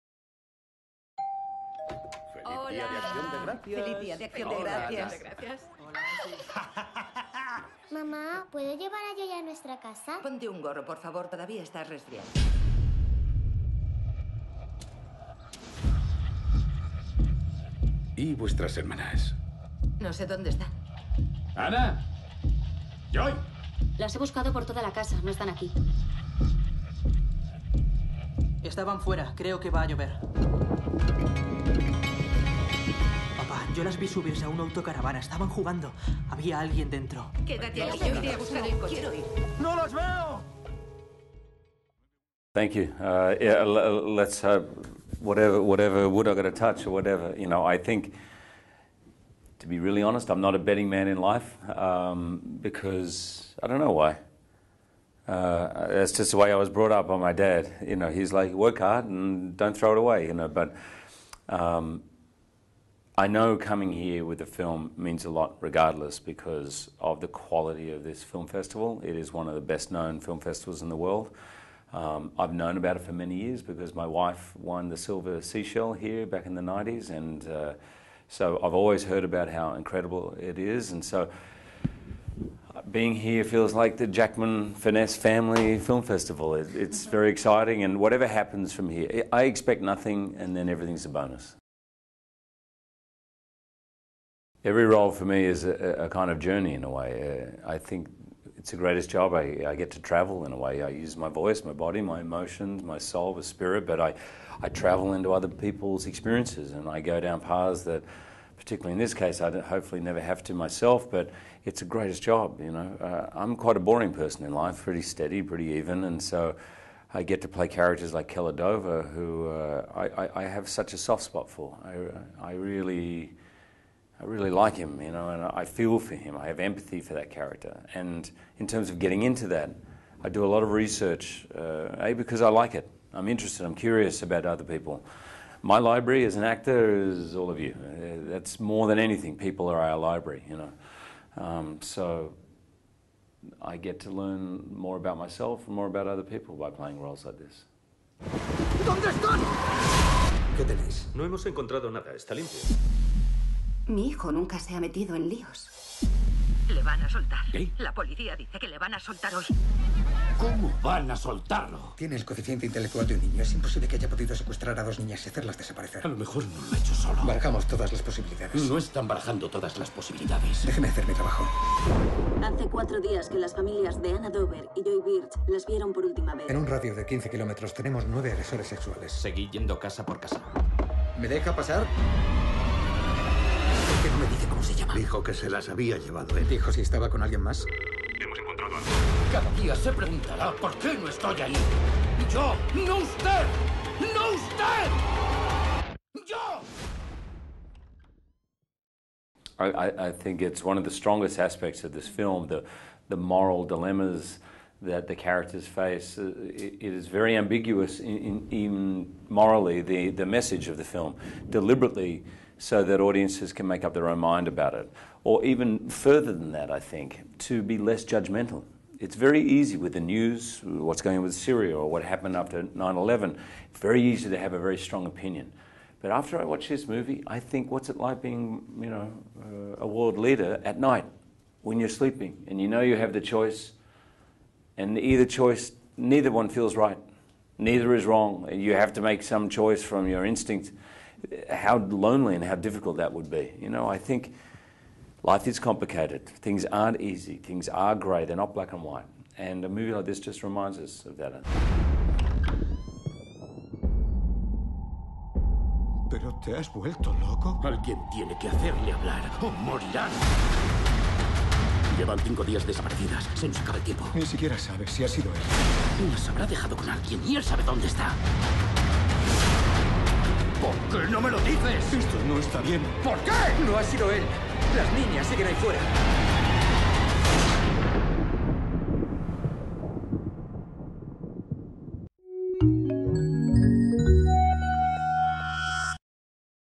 Hugh Jackman, un padre con lado oscuro en 'Prisioneros'. Cadena SER
El actor australiano interpreta a un padre cuya hija ha desaparecido y a la que busca con la ayuda de un detective (Jake Gyllenhaal). Este viernes se estrena 'Prisioneros', la cinta que Jackman presentó en el Festival de San Sebastián, un thriller con muchas lecturas sociales que dirige el canadiense Denis Villeneuve y que suena para los Oscar